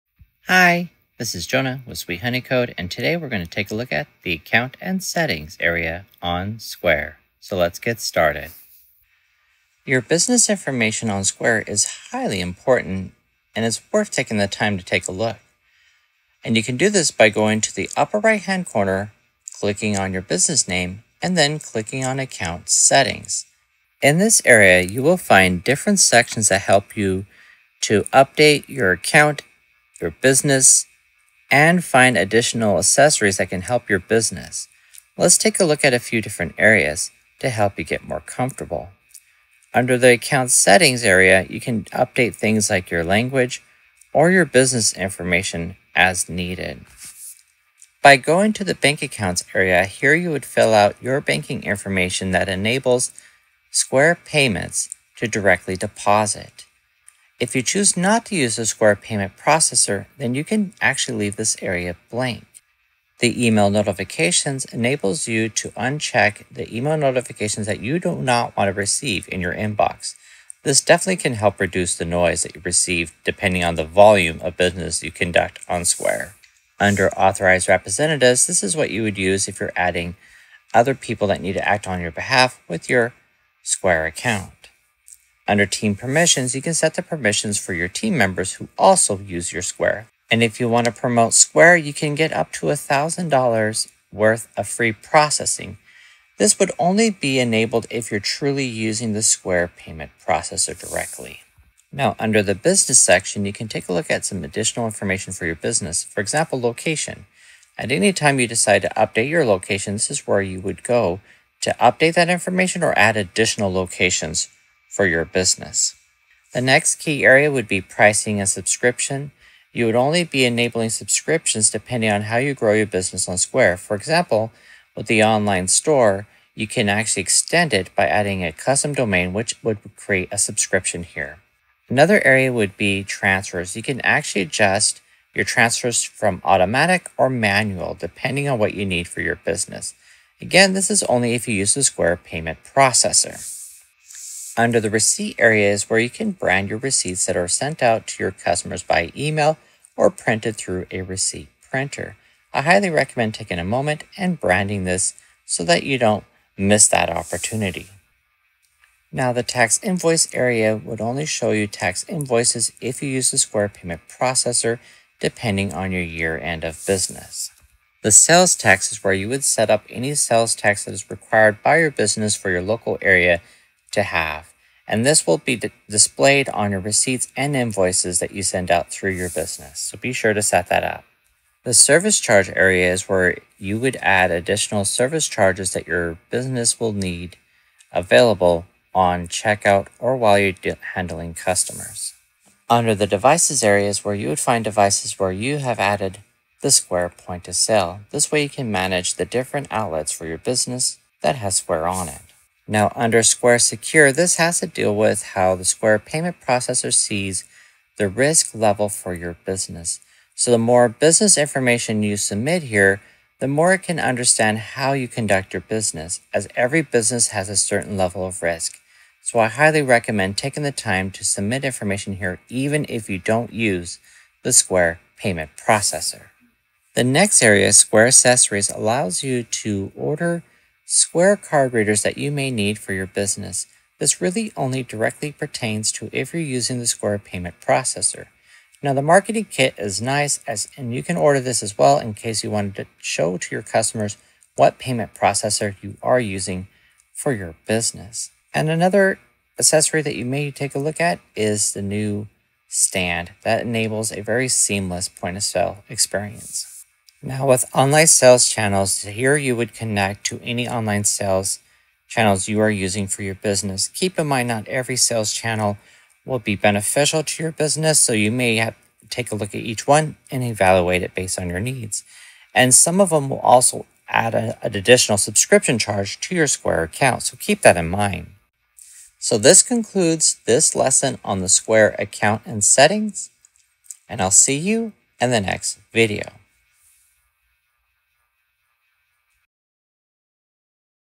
Account and Settings on Square
In this video I talk about the dashboard on Square.